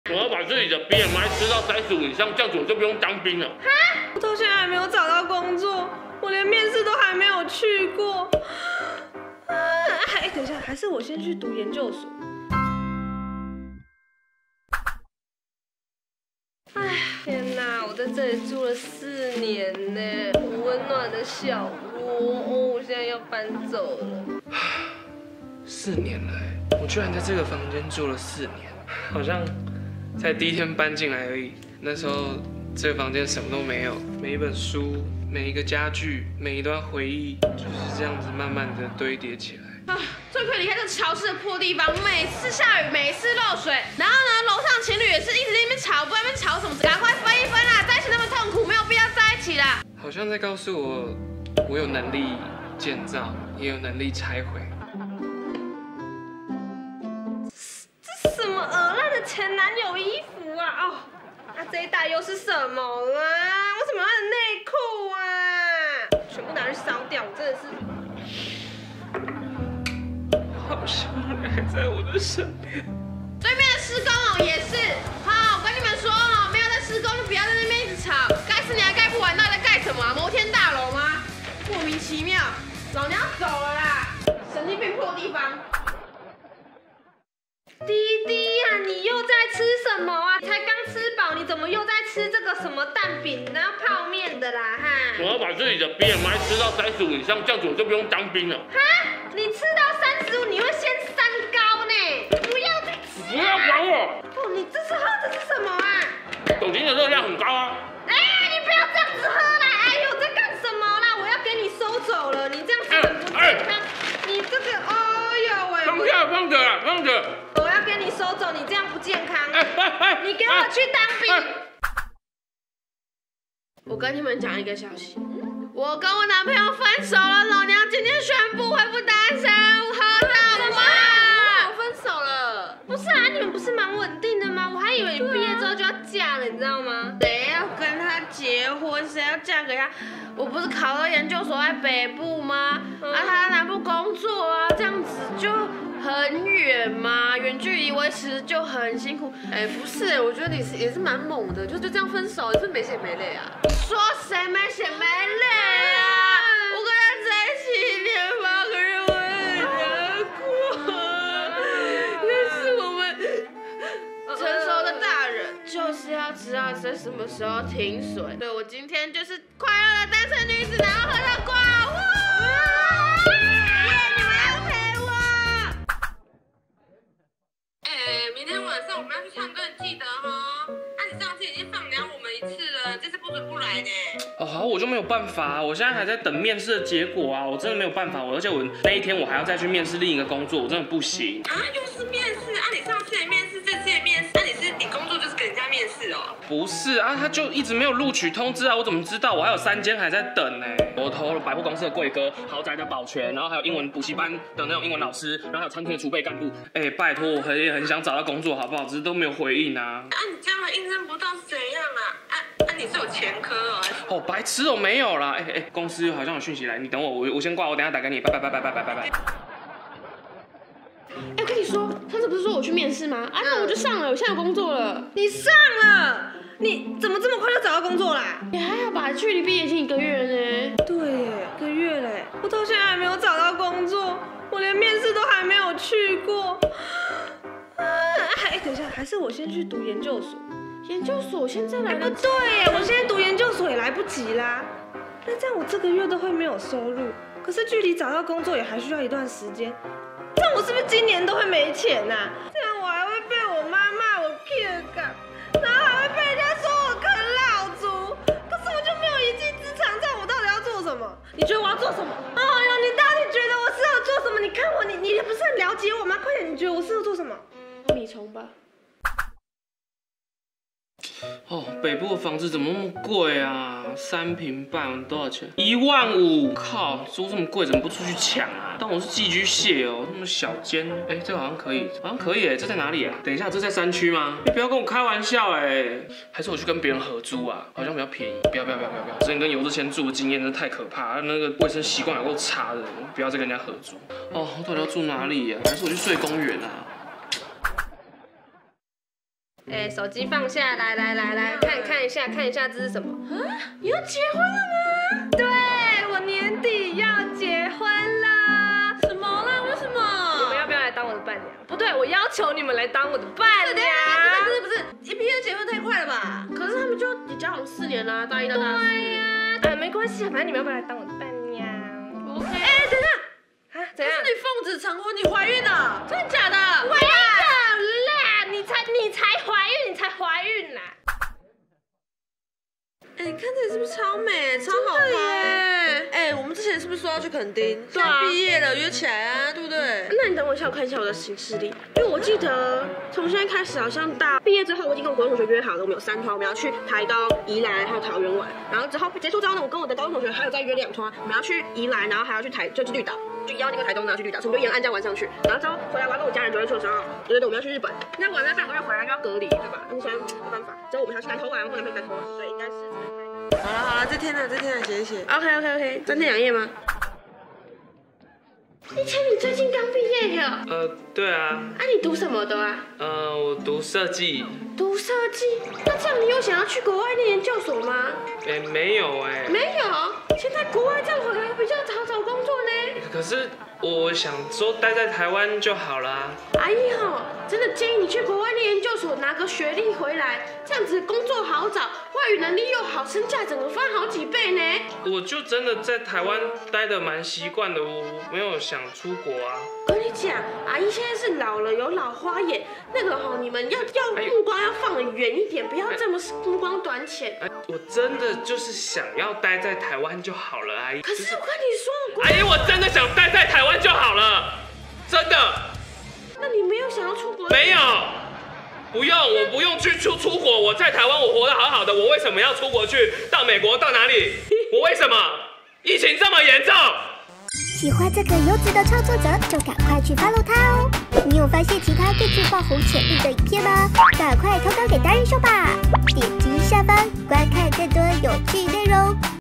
【那些大學剛畢業的各種同學 分手潮？啃老？焦慮爆發！】｜@tongsyue
你是哪一種呢？ (靜香編)